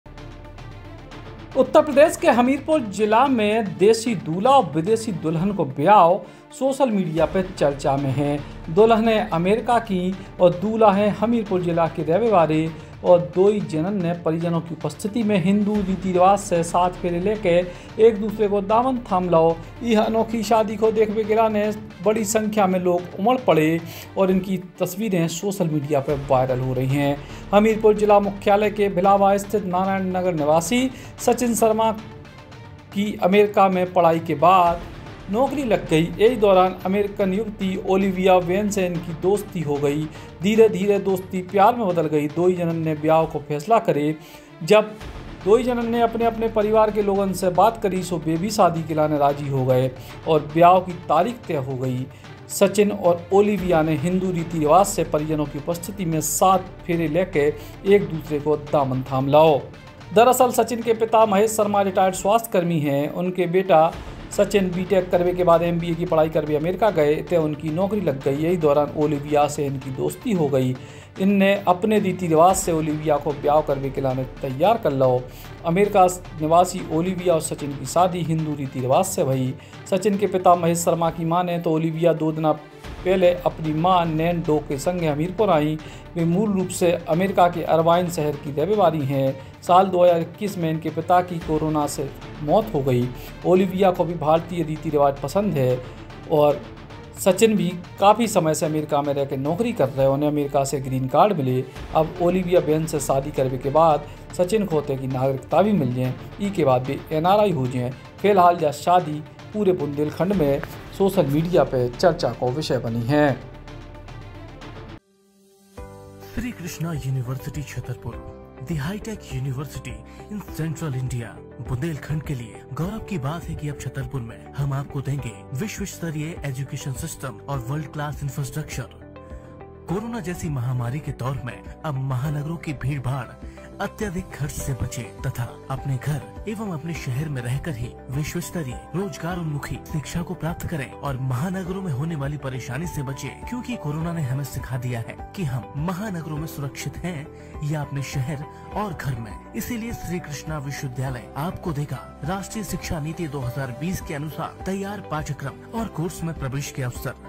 बुंदेलखंड में देशी दूल्हा और विदेशी दुल्हन की शादी की गजब चर्चा
BUNDELI BAUCHAAR

Suggested Pages :-